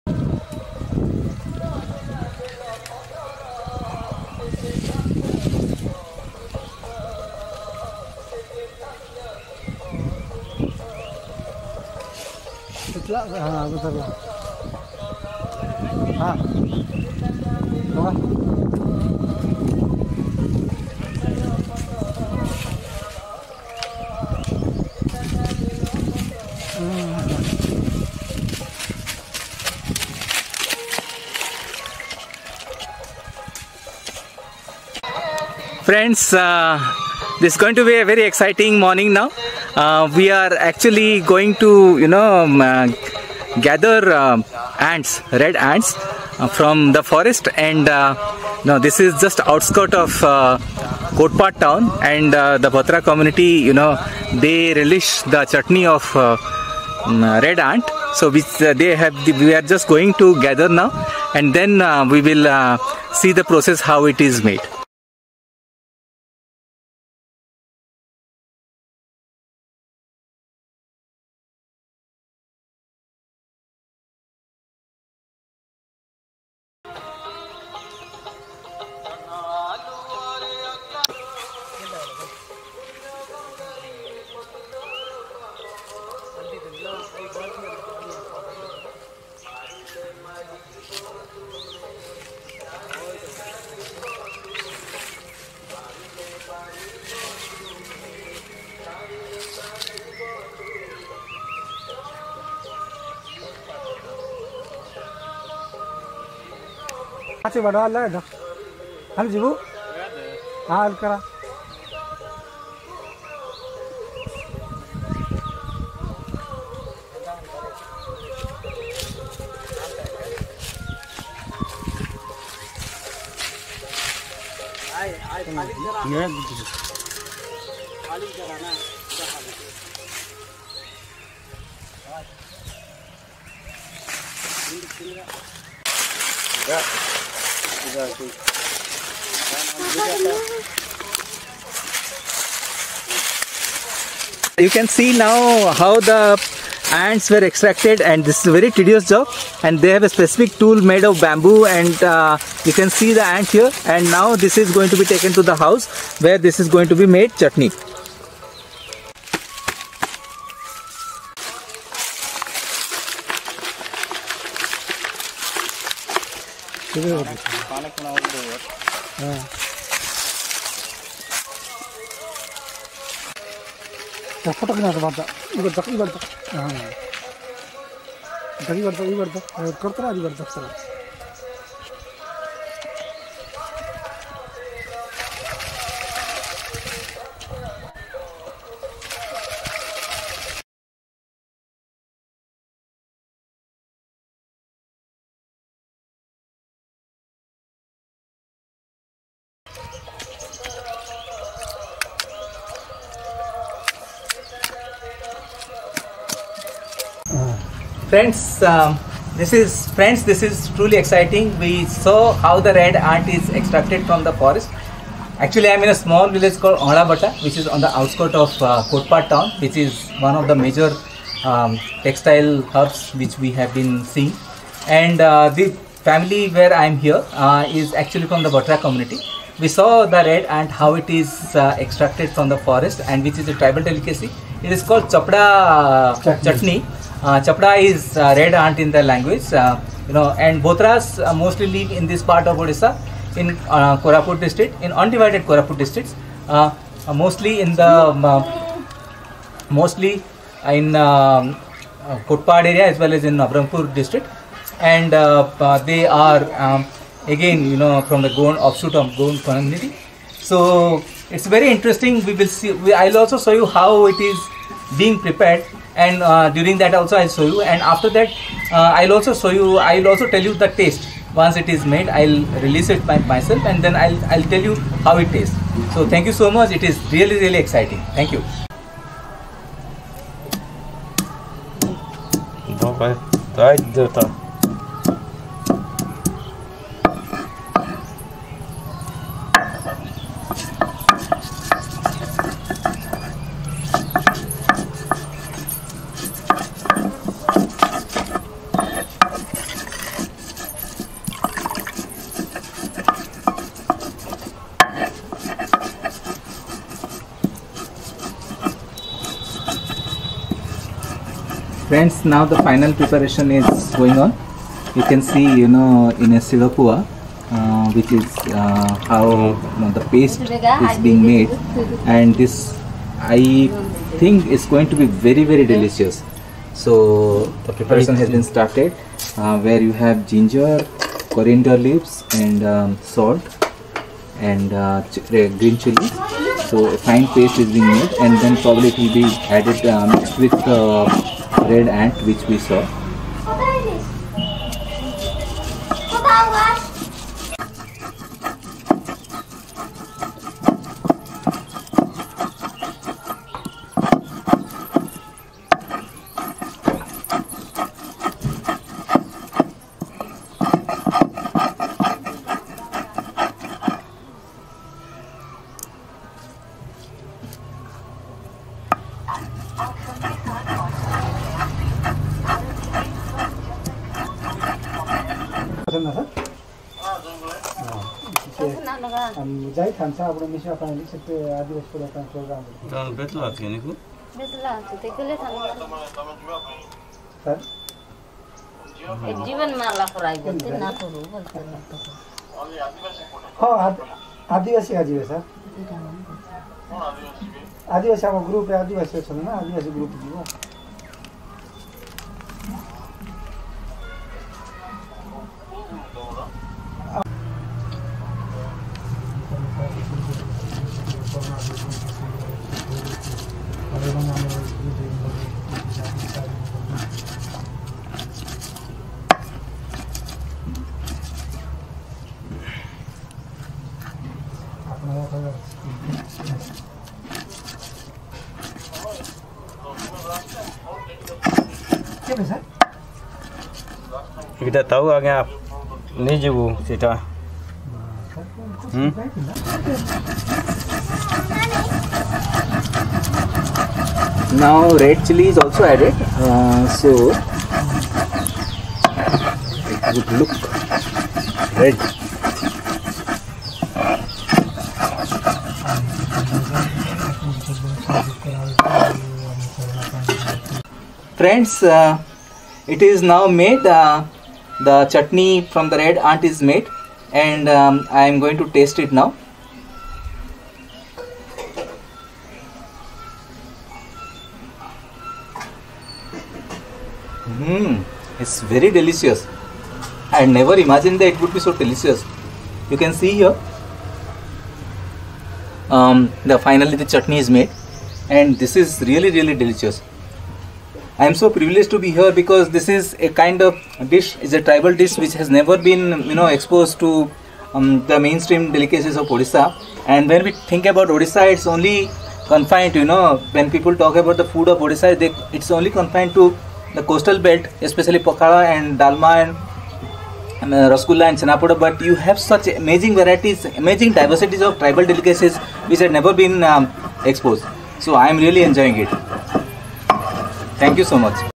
Chapada Chutney - A Culinary Journey in Kotpad
Odisha is a land of cultural diversity which is also reflected in its food heritage. This video explores Chapada chutney, a tribal delicacy from the Kotpad region in Koraput. The main ingredient of this delicacy is a red ant, locally called Chapada.